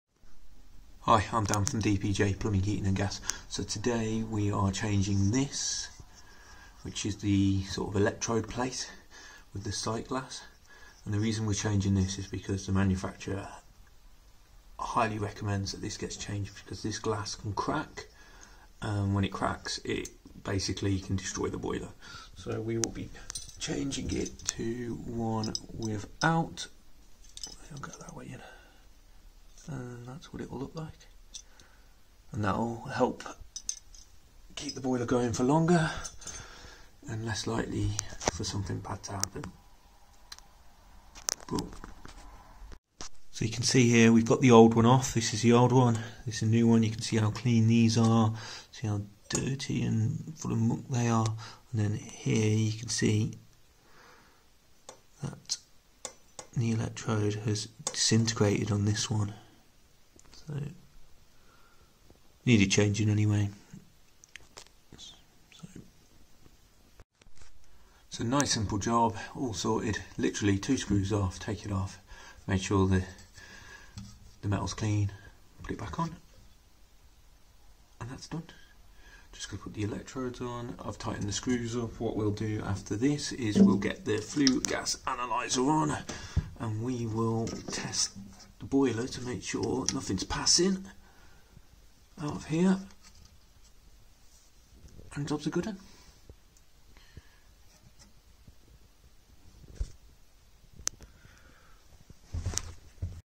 Worcester Electrode Plate Replacement
We replaced the electrode plate on a Worcester Greenstar 40CDi because it had the original glass view. The manufacturer recommends that all these electrode plates be changed to the solid plate design as the glass screen is known to crack and cause damage to the boiler.